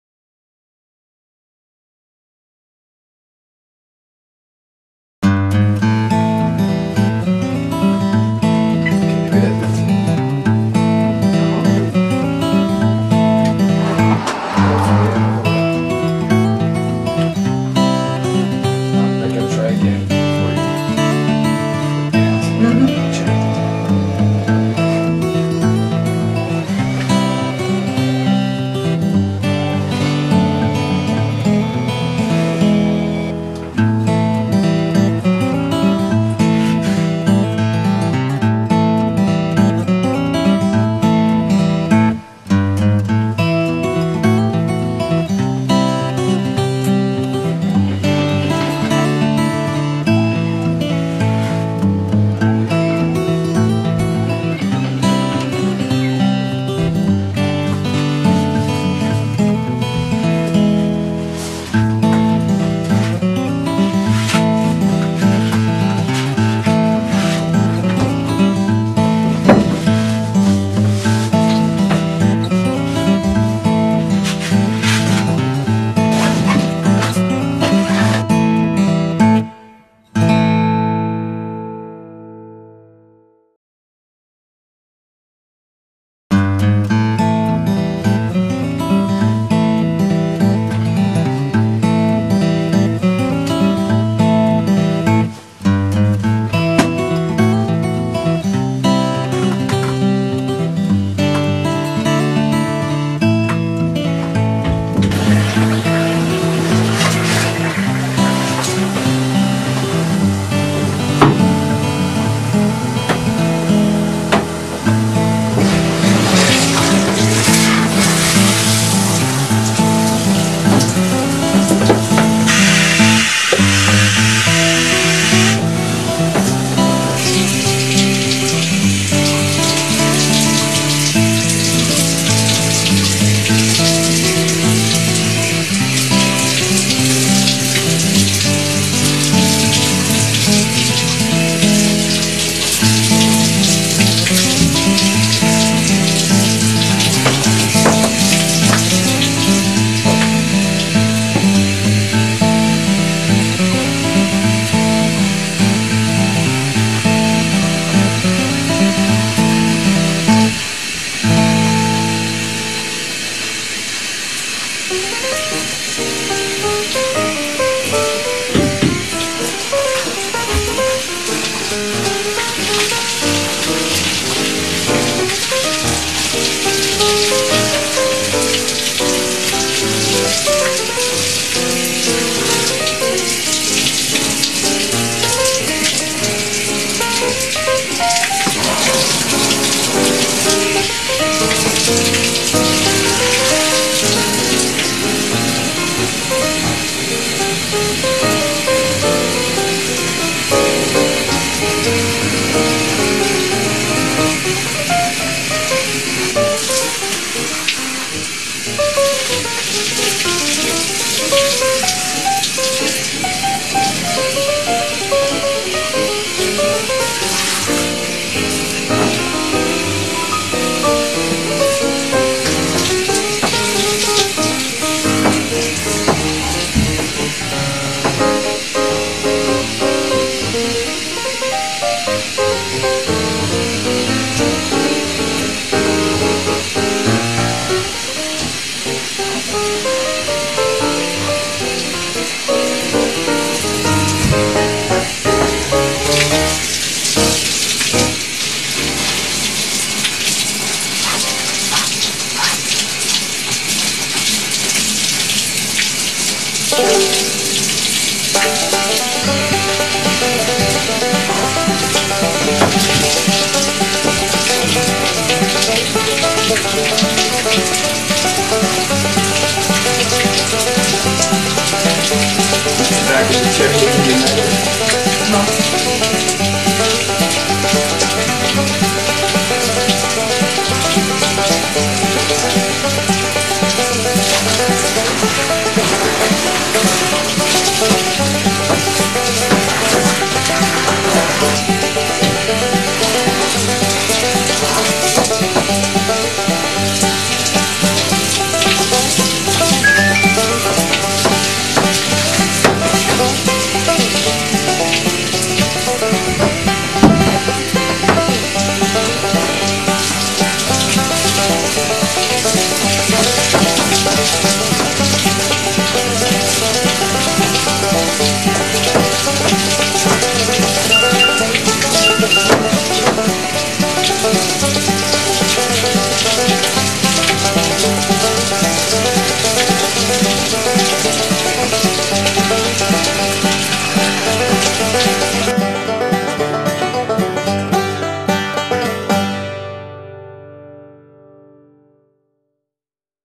Quadriplegic Shower Care Part 1
This is part 1 of me during all of my bathroom cares. I am a C5-C6 quadriplegic. This was in 2007.

GOOGLE   Chris Colwell